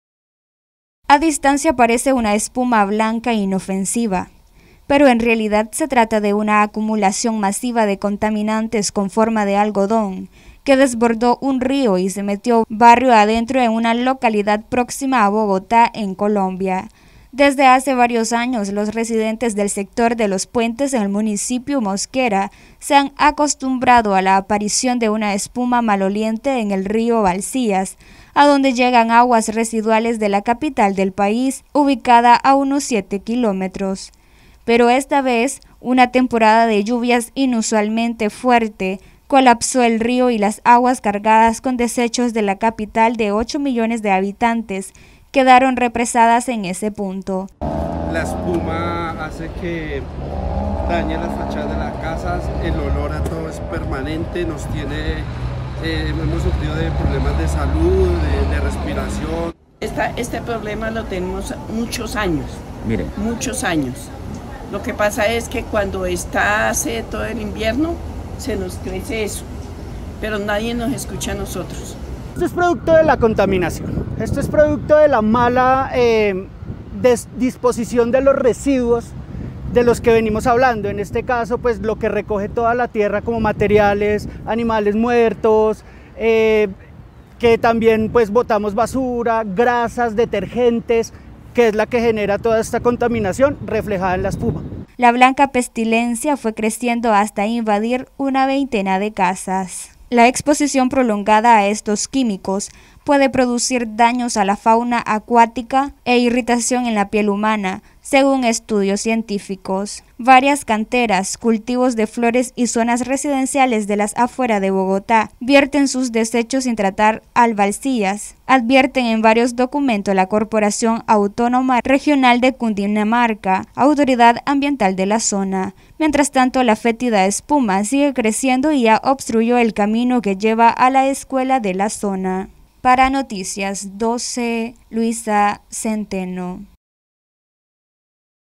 La contaminación ambiental crece como espuma en Colombia
InternaiconalesLa contaminación ambiental crece como espuma en las afueras de Bogotá, Colombia